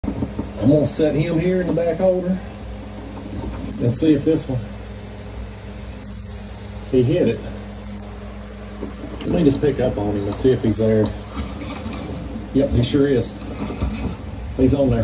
I Can’t sleep so I painted a bluegill pattern squarebill Cranker. It’s a bass killer!
Watching kayak catfish and painting a lure late at night. fish bait lures BASS tournament catch and release